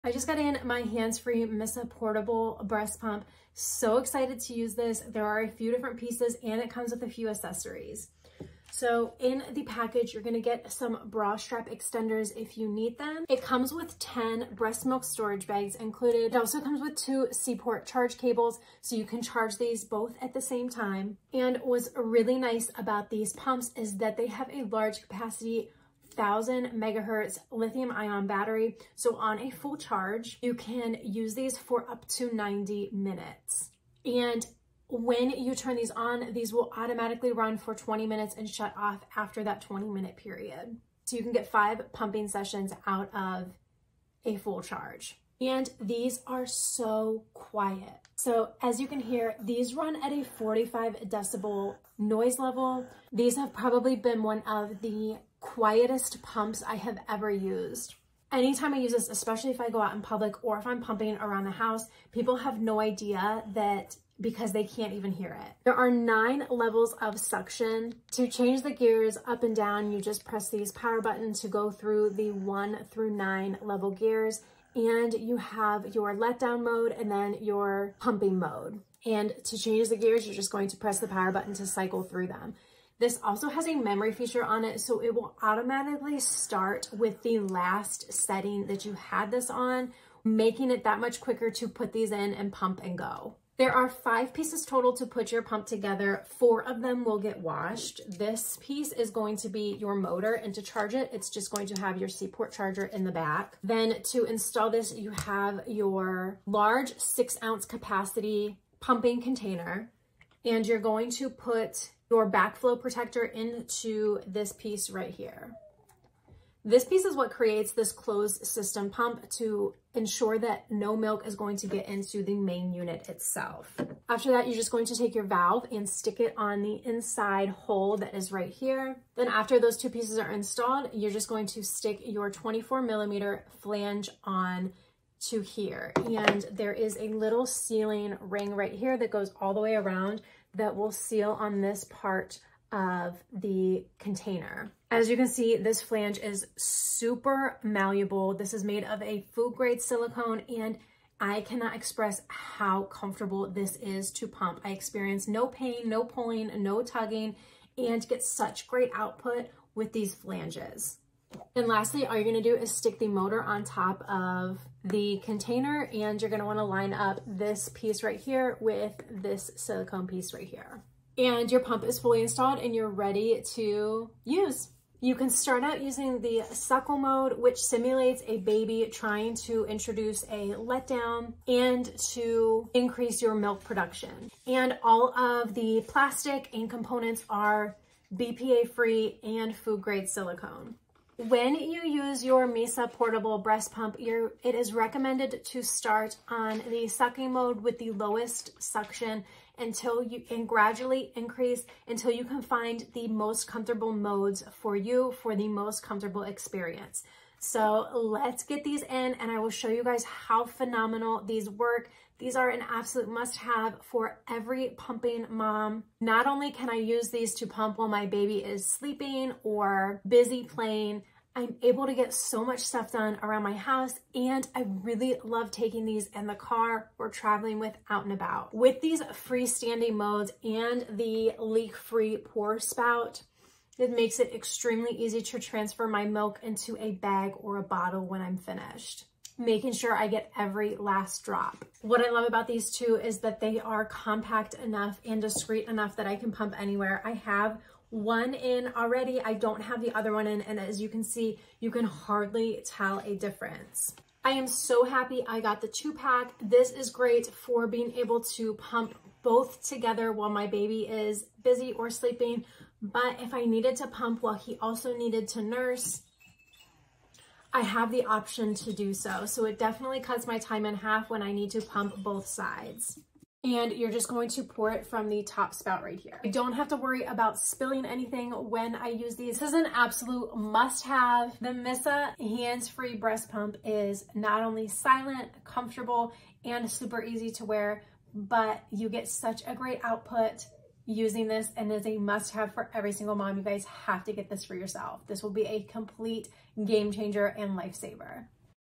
MISSAA S18 Wearable Breast Pump-Let mothers enjoy breastfeeding life！
Mom experienced it firsthand, she'll let you know more about this breast pump, and she highly recommends it !